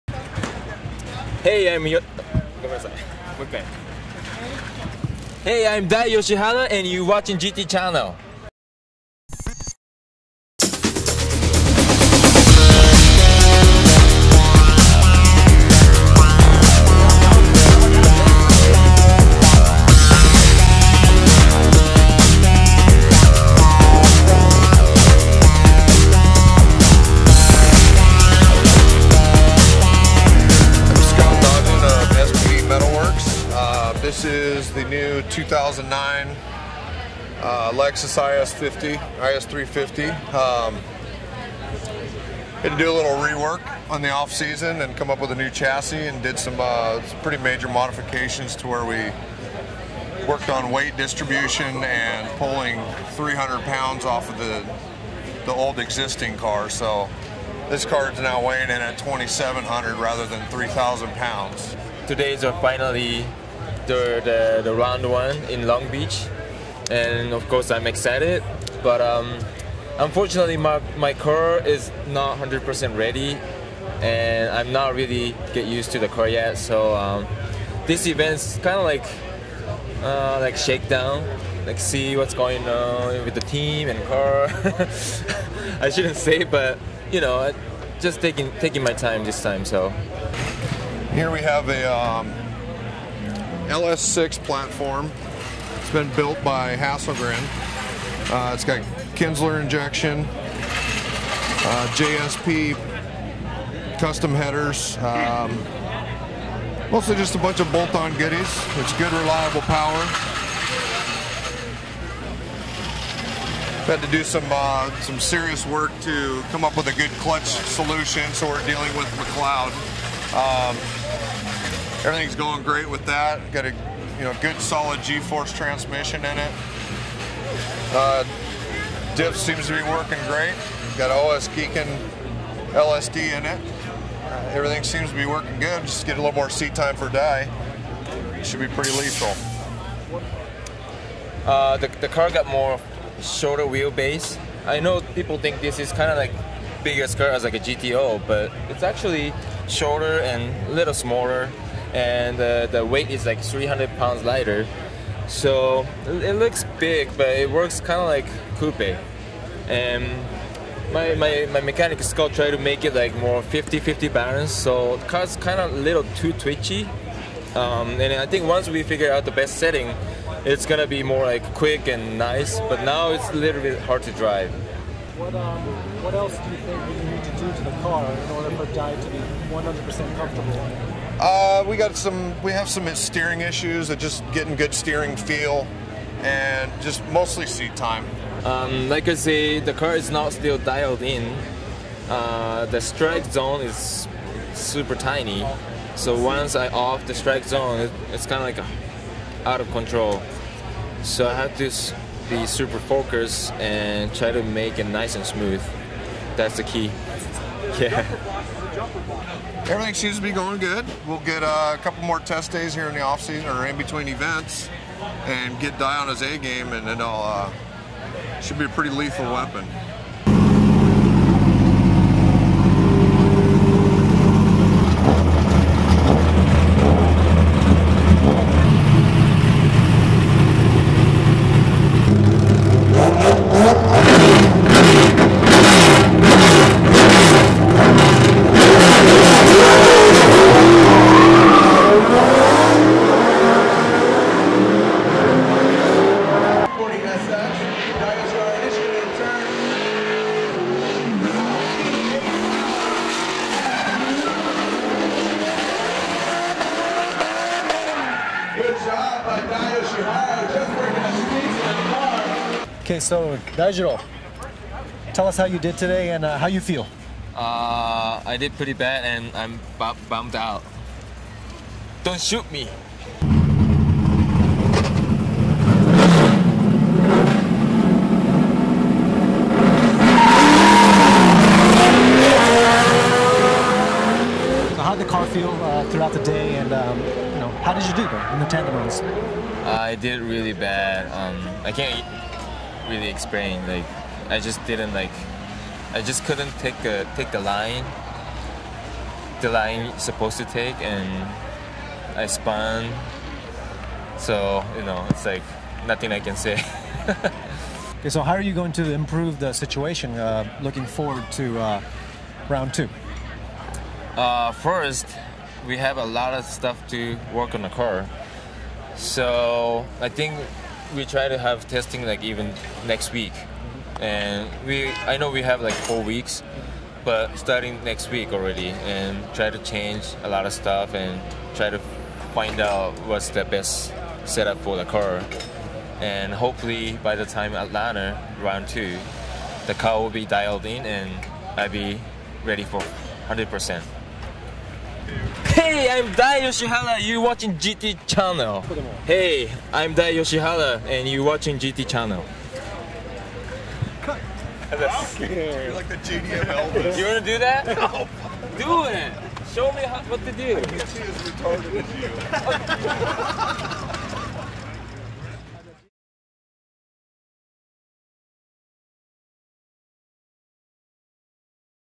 Dai Yoshihara's 09 Lexus IS Drift Car in Detail at Formula D LB
to check out his 2010 ride, the V8 powered Nissan S13! In this video we go in depth to cover Daijiro Yoshihara's Discount Tire Team Falken Lexus IS350 at Formula Drift 2009 Long beach.